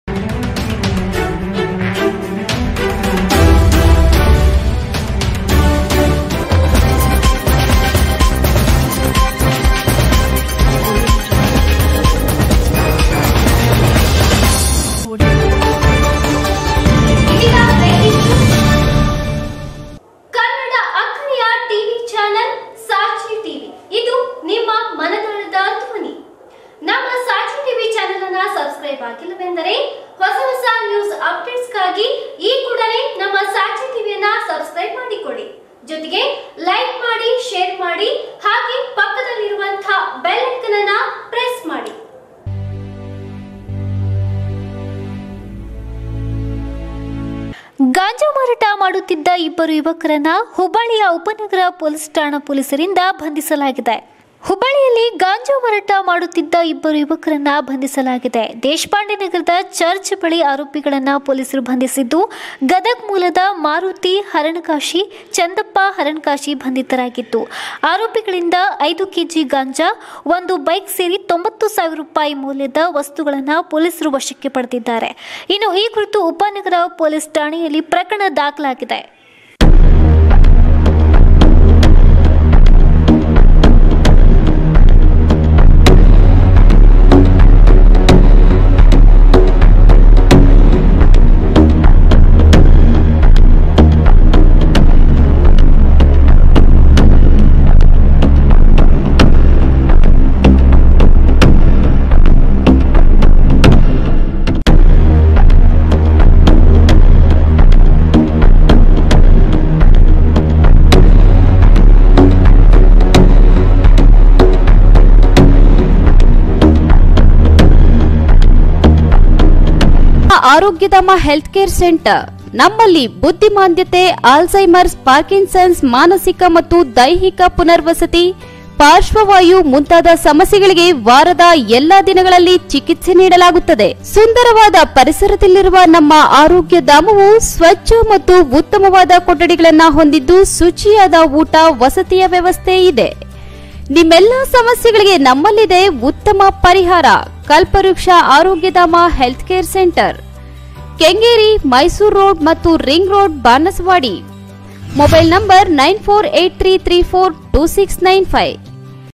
ಹುಬ್ಬಳ್ಳಿಯಲ್ಲಿ ಗಾಂಜಾ ಮಾರಾಟ ಮಾಡುತ್ತಿದ್ದ ಇಬ್ಬರು ಯುವಕರ ಬಂಧನ!! |sachi tv
ಹುಬ್ಬಳ್ಳಿ

ಹುಬ್ಬಳ್ಳಿಯಲ್ಲಿ ಗಾಂಜಾ ಮಾರಾಟ ಮಾಡುತ್ತಿದ್ದ ಇಬ್ಬರು ಯುವಕರ ಬಂಧನ

ಹುಬ್ಬಳ್ಳಿಯ ಉಪನಗರ ಪೊಲೀಸ್ ಠಾಣಾ ಪೊಲೀಸರಿಂದ ಯುವಕರ ಬಂಧನ

ದೇಶಪಾಂಡೆ ನಗರದ ಚರ್ಚ್ ಬಳಿ ಆರೋಪಿಗಳನ್ನು ಬಂಧಿಸಿರೋ ಪೊಲೀಸರು

ಗದಗ ಮೂಲದ ಮಾರುತಿ ಹರಣಕಾಶಿ, ಚಂದಪ್ಪ ಹರಣಕಾಶಿ ಬಂಧಿತರು

ಆರೋಪಿಗಳಿಂದ ಐದು ಕೆಜಿ ಗಾಂಜಾ, ಒಂದು ಬೈಕ್ ವಶ

ಬೈಕ್ ಸೇರಿ 90 ಸಾವಿರ ರೂಪಾಯಿ ಮೌಲ್ಯದ ವಸ್ತುಗಳನ್ನು ವಶಕ್ಕೆ ಪಡೆದಿರೋ ಪೊಲೀಸರು

ಉಪನಗರ ಪೊಲೀಸ್ ಠಾಣೆಯಲ್ಲಿ ಪ್ರಕರಣ ದಾಖಲು